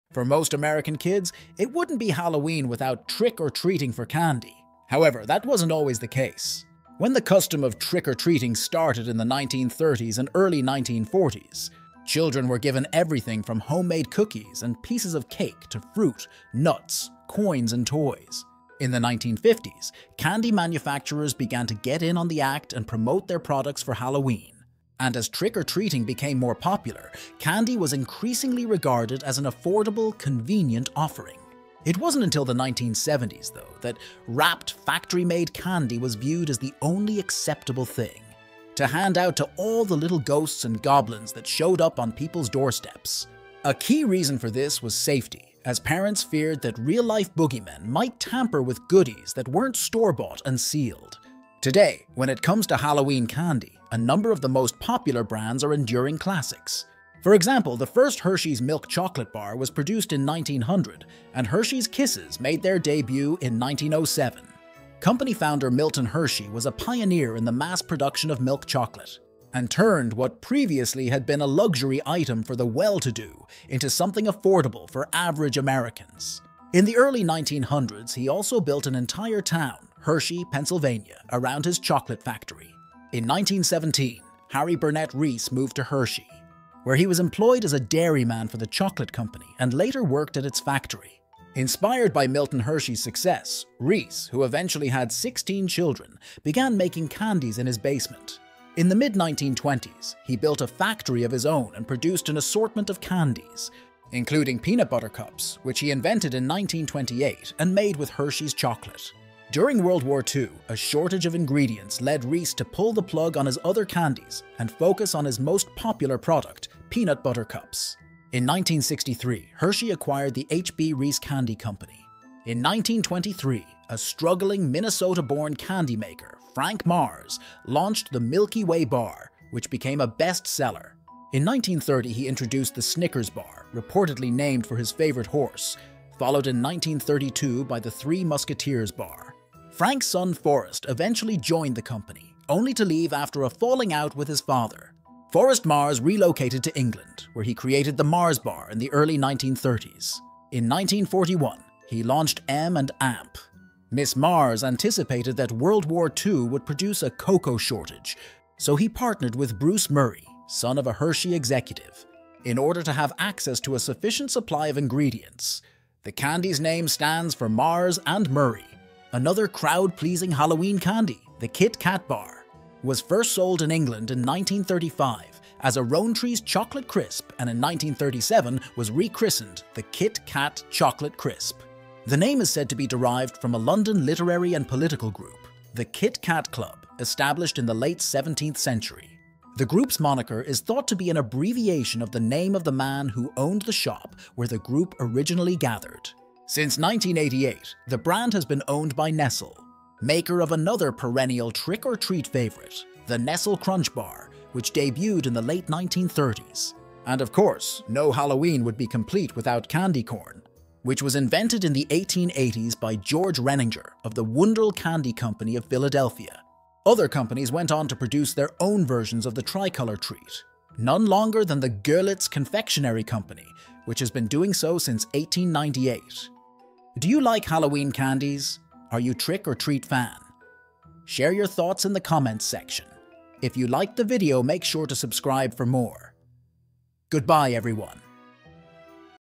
The Haunted History of Halloween Candy - Let's Go Back in Time
The Haunted History of Halloween Candy.

For most American kids, it wouldn’t be Halloween without trick-or-treating for candy. But that wasn’t always the case.